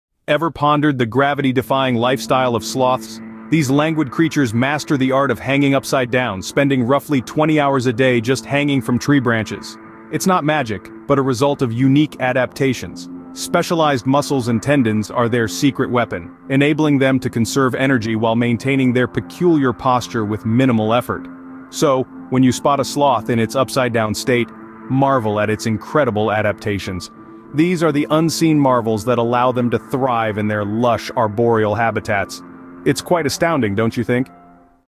Gravity-Defying Sloths: A Short Exploration
In this captivating video, we delve into the intriguing world of sloths and their remarkable adaptations that enable them to thrive while hanging upside down for the majority of their lives. From their unique anatomy to their slow-paced lifestyle, we uncover the secrets behind these fascinating creatures.

Join us as we uncover the evolutionary wonders of sloths and how they have perfected the art of life in the treetops. Discover why sloths move so slowly, how they find food in their forest canopy homes, and why their upside-down lifestyle is essential for their survival.

If you're curious about nature's most laid-back tree-dwellers, don't miss this deep dive into the world of sloths.

Lord of the Facts - Once Click to Reveal them all!